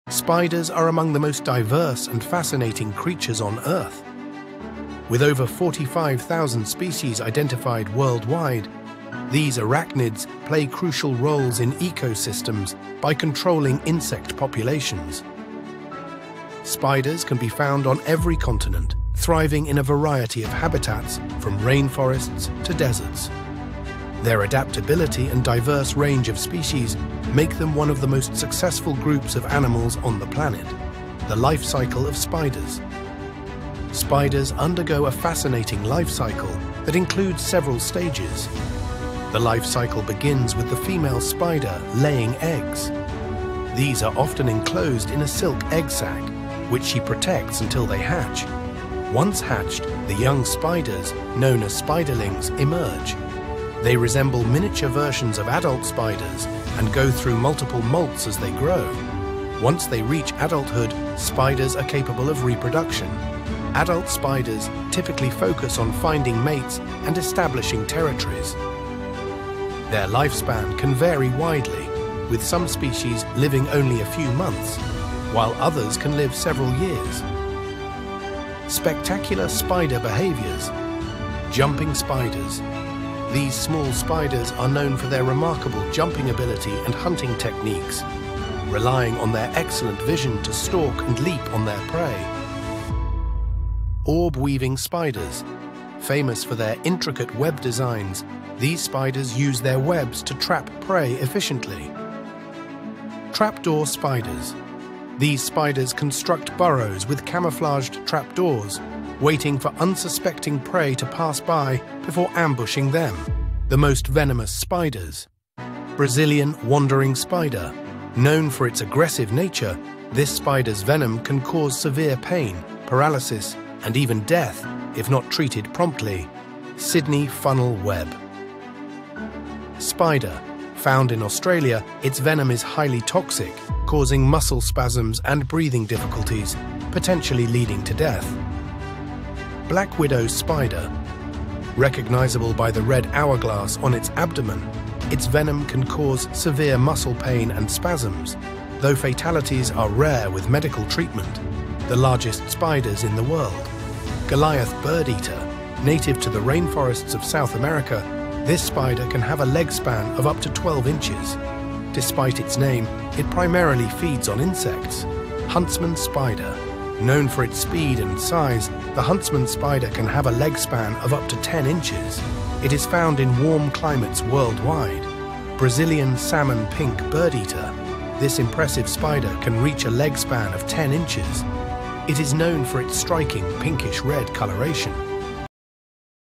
Exploring the World of Spiders: From the Most Venomous to the Largest Species
🕷️ Dive into the captivating world of spiders with us! From the most venomous to the largest species, this video will change the way you see these incredible arachnids.

🔍 In this video:

The Life Cycle of Spiders Unveiled
Spectacular Spider Behaviors Revealed!
Discover the Most Venomous Spiders 🕷️💀
Meet the Giants: The Largest Spiders in the World 🌍🕷️

Whether you're an arachnophile or just curious, there's something for everyone in this exploration of spiders' secrets.

Timecodes:
00:00 - Intro
00:31 - The Life Cycle of Spiders
01:19 - Spectacular Spider Behaviors
01:54- The Most Venomous Spiders
02:32 - The Largest Spiders in the World